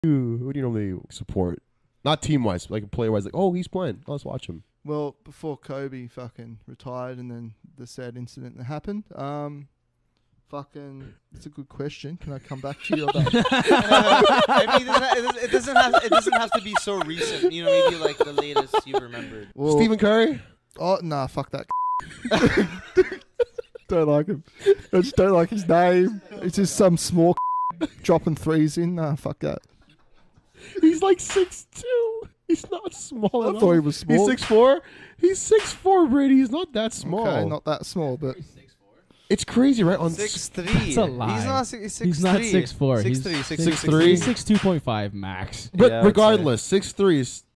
Markus Paterson and Jameson Blake laugh uncontrollably as Brady talks about Stephen Curry!
Clip from EP 8, full episode on our channel